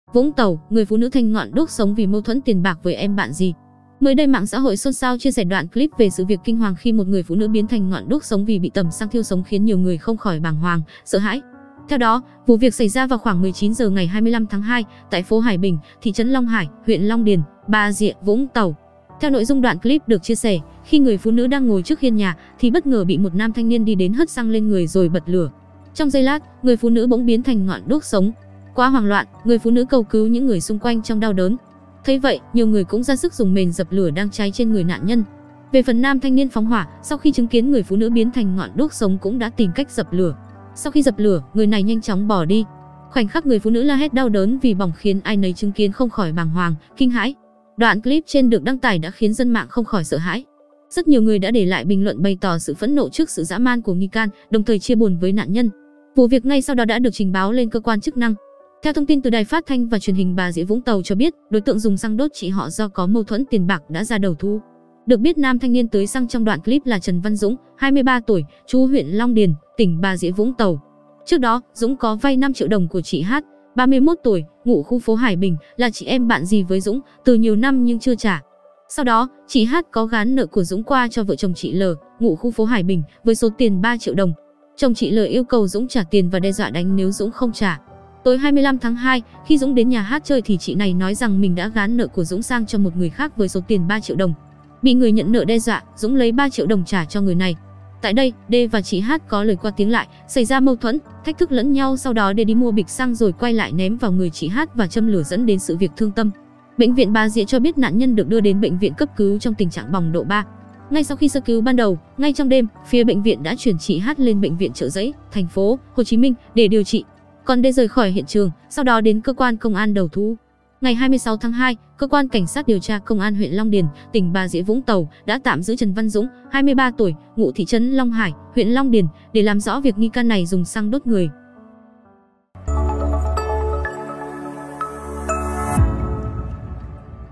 Vũng Tàu: Người phụ nữ thành ngọn đuốc sống vì mâu thuẫn tiền bạc với em bạn dì
Mới đây MXH xôn xao chia sẻ đoạn clip về sự việc kinh hoàng khi 1 người phụ nữ biến thành "ngọn đuốc sống" vì bị tẩm xăng thiêu sống khiến nhiều người không khỏi bàng hoàng, sợ hãi.
Theo đó, vụ việc xảy ra vào khoảng 19h ngày 25/2, tại phố Hải Bình, thị trấn Long Hải, huyện Long Điền, Bà Rịa - Vũng Tàu.